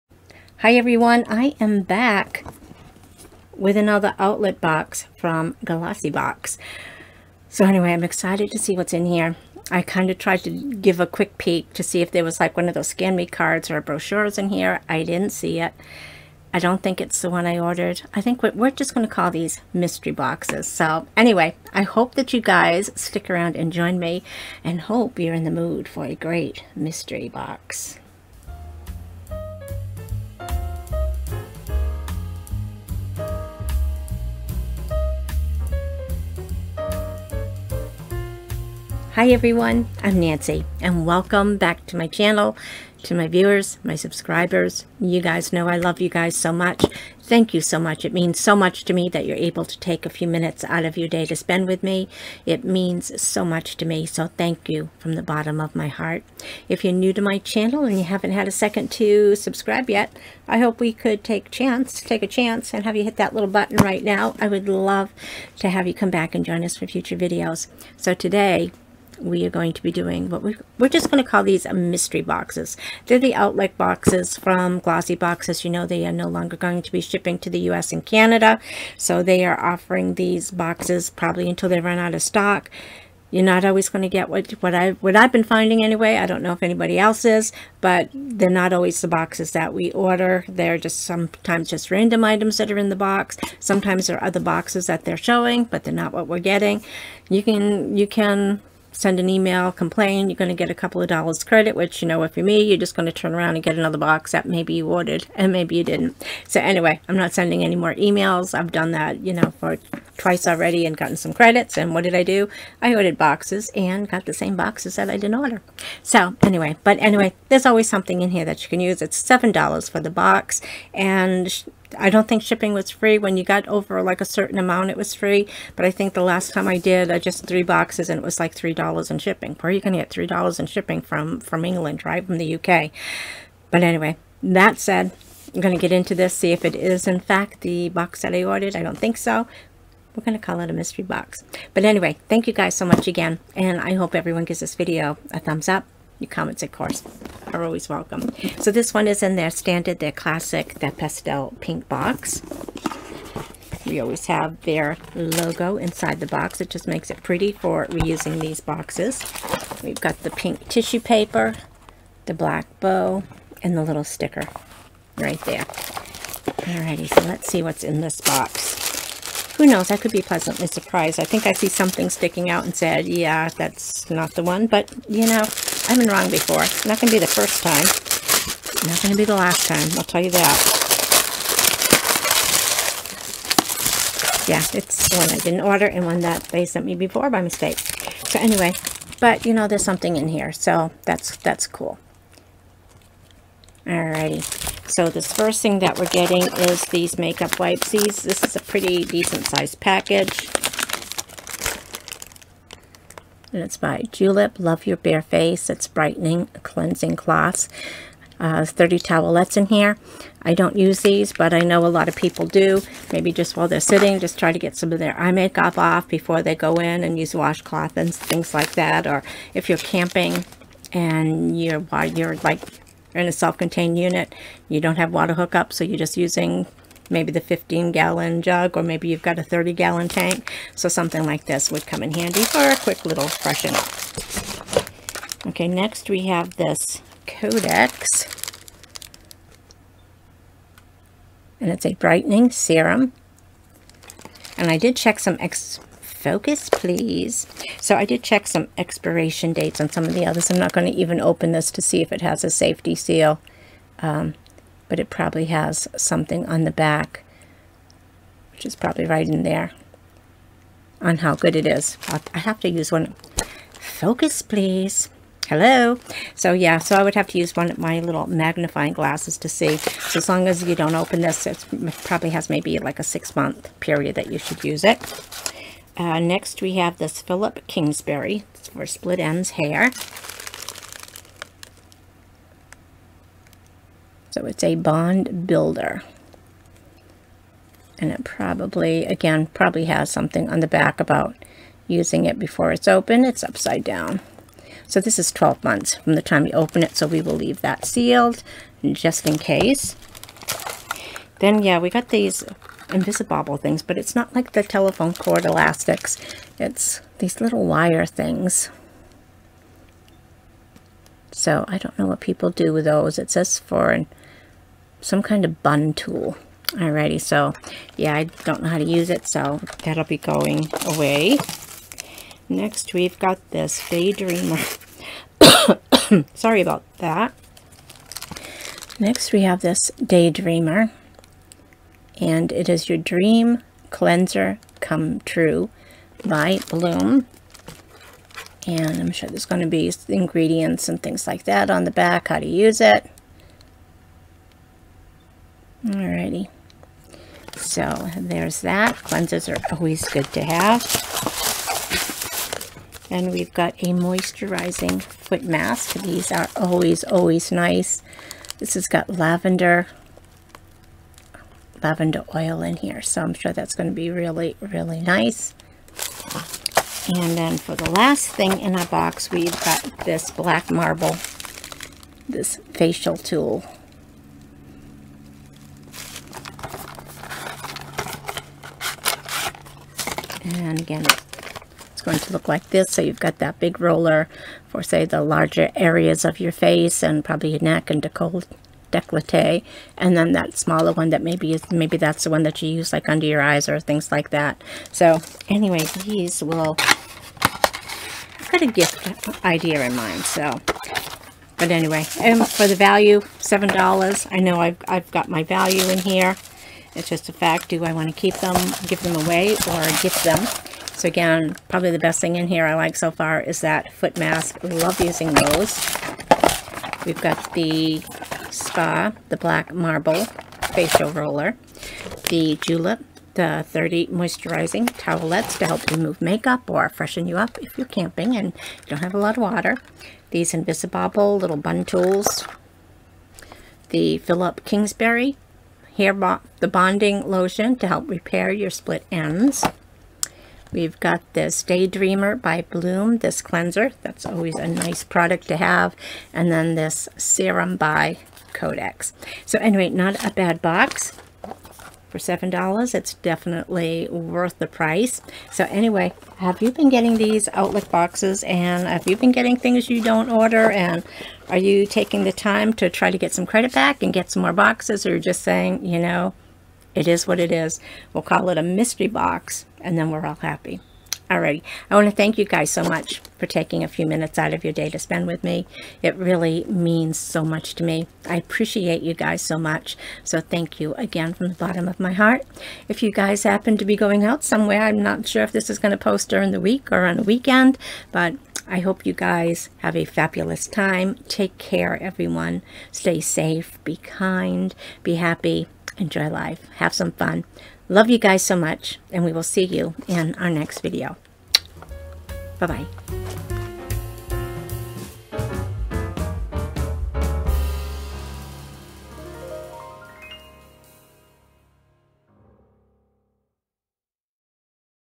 Glossy Box Outlet 7
Glossy Box!!

Outlet Sale: This box was $7.00 **
Value at least $75.00

**These boxes are hit or miss, sometimes you get the box you ordered, most times for me anyway, they keep sending boxes I did not order!

Wig and Dress by Temu

 XOXOXO

Nancy